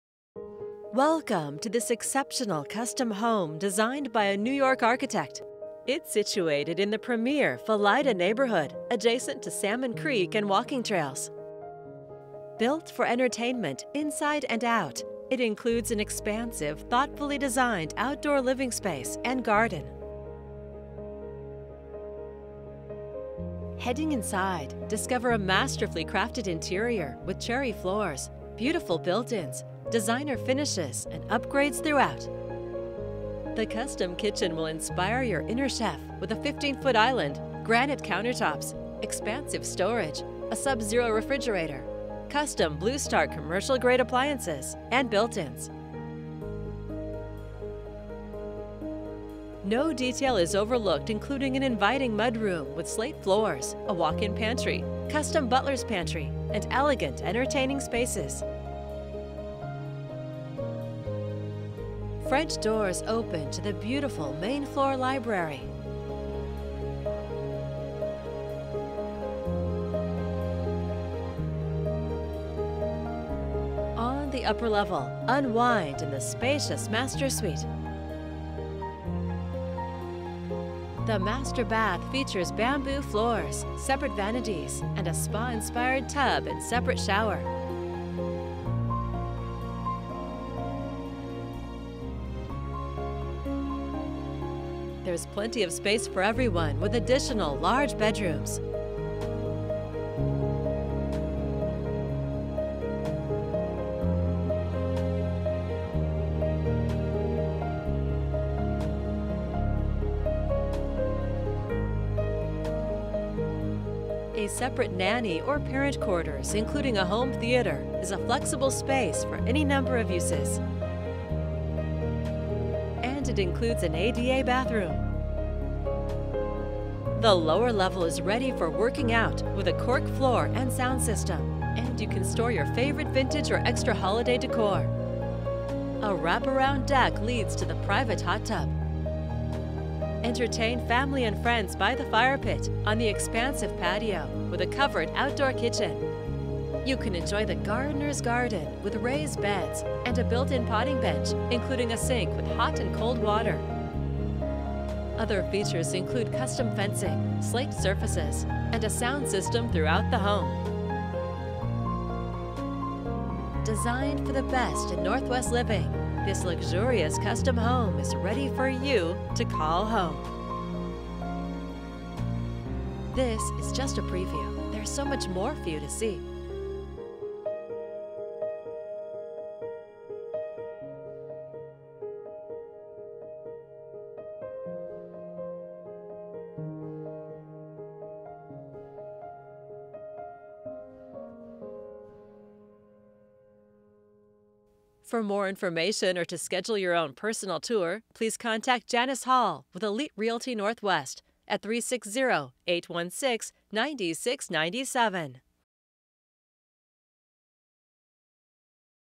Exceptional custom home in NW Vancouver ~ Video of 5113 NW 143rd St.
Gorgeous custom home by a New York architect, situated the highly desirable Felida neighborhood near Salmon Creek and walking trails. High end features and finishes throughout, including cherry hardwoods, surround sound, chef's kitchen. Home theater. Expansive outdoor living spaces, fire pit, outdoor kitchen. Contact Janice Hall with Elite Team NW.